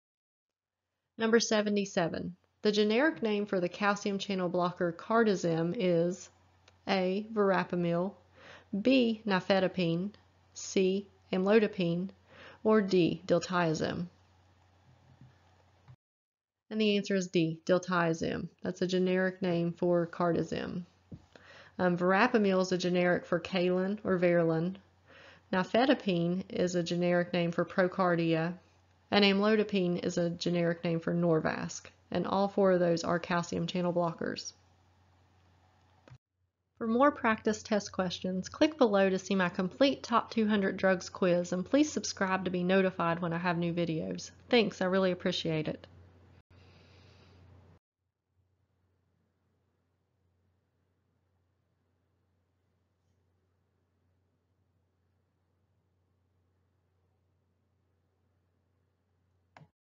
Top 200 Drugs PTCB Practice Test Question - generic name for calcium channel blocker Cardizem is
Top 200 Drugs Practice Test Question 77 - The generic name for the calcium channel blocker Cardizem is: Individual practice test questions from my Top 200 Drugs Quiz #1 Video based on my Top 200 Drugs Flashcards with Audio. For pharmacy students, pharmacy technicians, and nursing students preparing for NAPLEX, PTCB PTCE, or NCLEX.